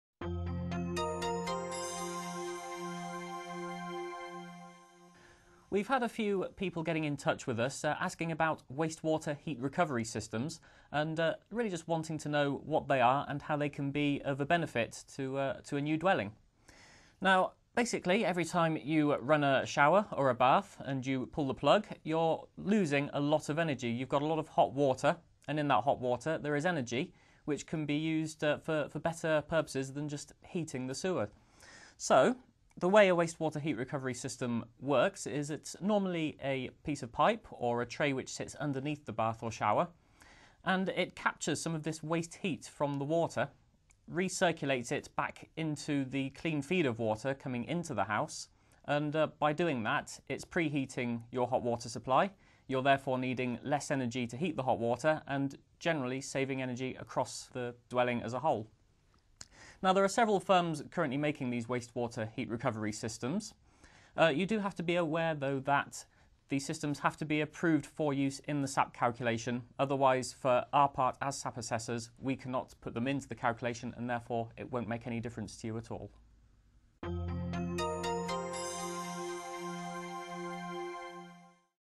Energist UK - Waste Water Heat Recovery System
This video explores the world of waste water heat recovery systems. It describes how they work, what benefits they can bring to a home and also how they can impact on your SAP assessment.